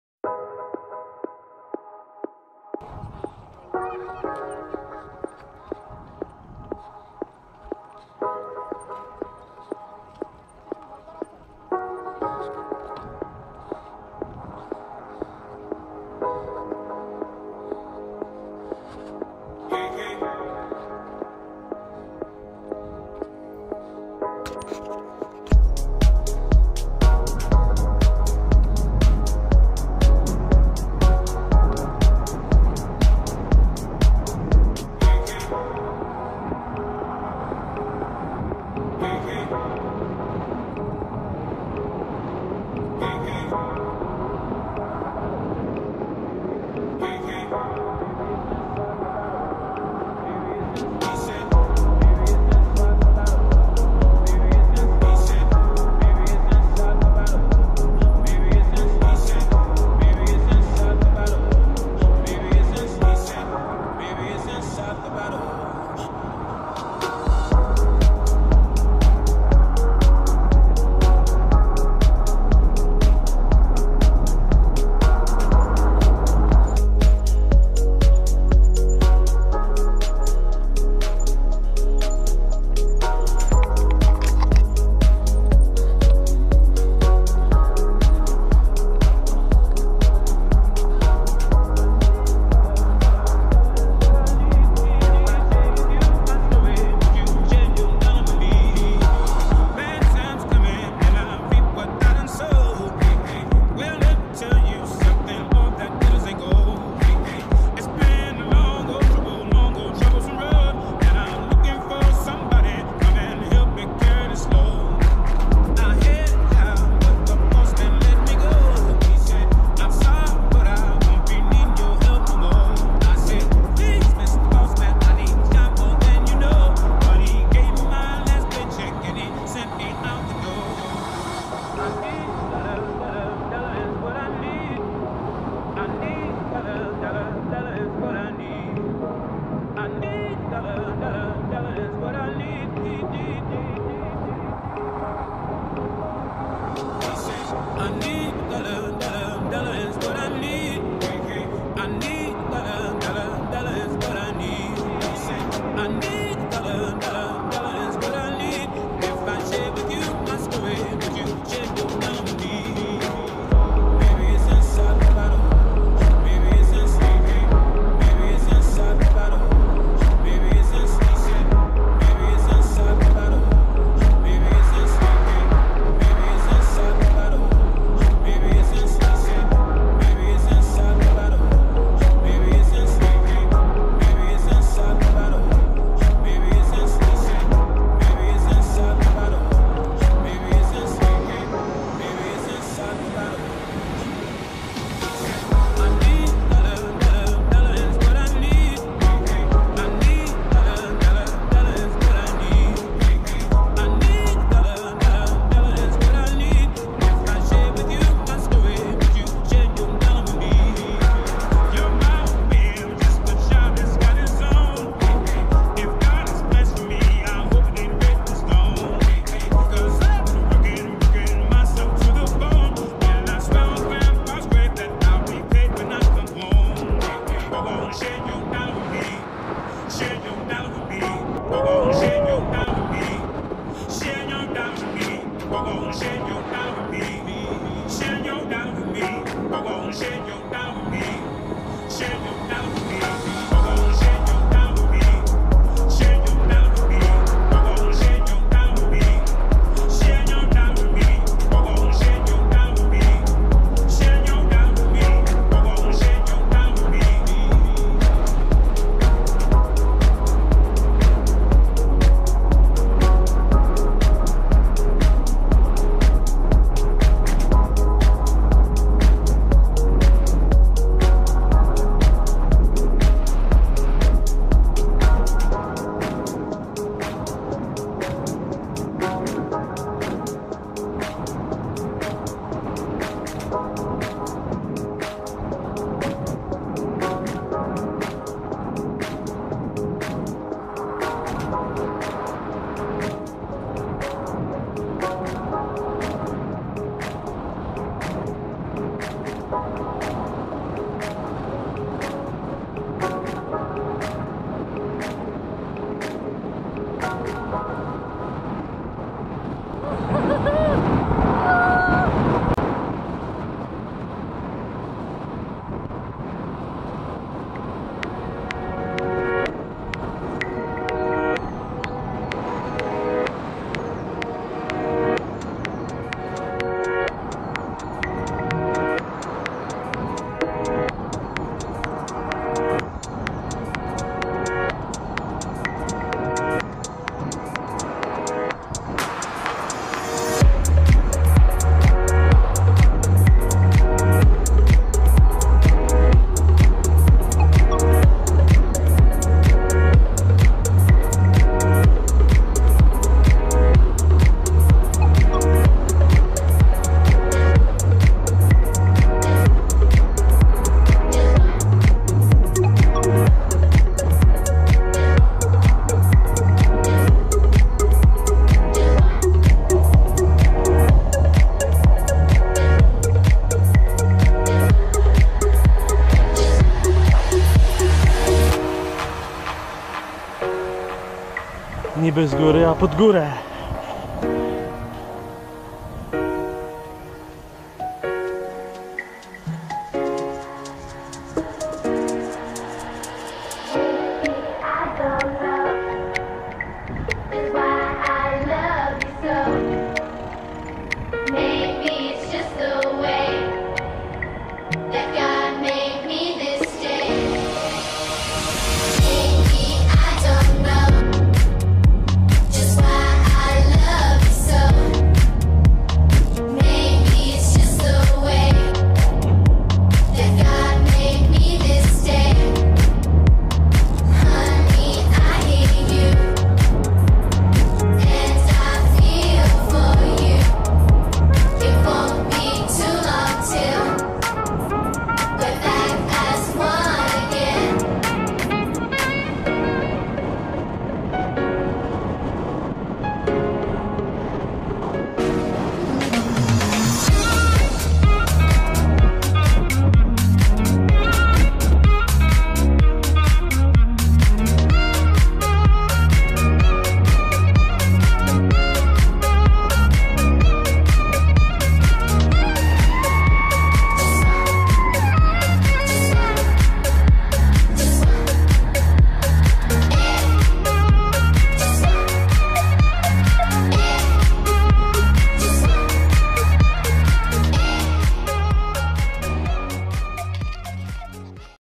Monsterroller Góra Żar - Anomalia Grawitacyjna czyli niby z góry a pod ...
Zjazd hulajnogą terenową ( monsterroller ) z Góry Żar w Międzybrodziu Żywieckim.

Trasa dłuższa - asfaltowa

Anomalia grawitacyjna w Karpaczu – zjawisko rejestrowane w Karpaczu Górnym na ul. Strażackiej, polegające na samodzielnym przemieszczaniu się obiektów kolistych i cieczy pozornie w górę po szosie. Zjawisko jest przykładem złudzenia optycznego - w rzeczywistości szosa obniża się po zboczu doliny, mimo że prowadzi w górę rzeki. Jednak niektórzy uznają to za rzeczywistą anomalię i wierzą, iż w tym miejscu przyciąganie ziemskie jest o 4% słabsze.